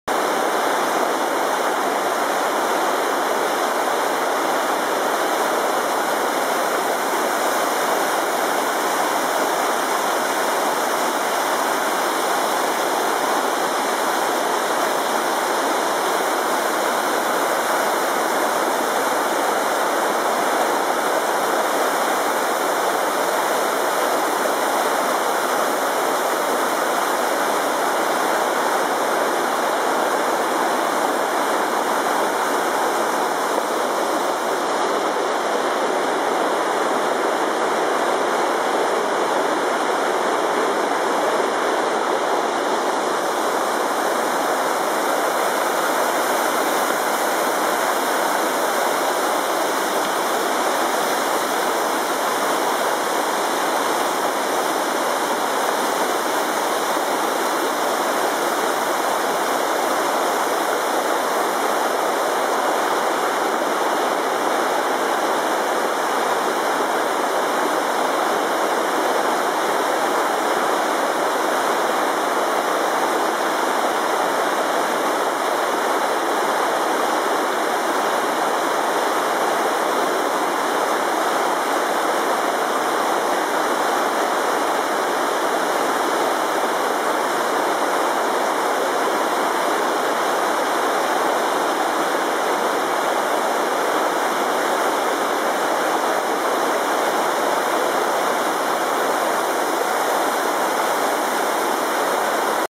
Carbon River in Mt Rainier National Park
This is one of the seven bridges across the Carbon River's various braids that enable a person to get to Chenuis Falls. I made the video because the still pictures just weren't capturing the raging river.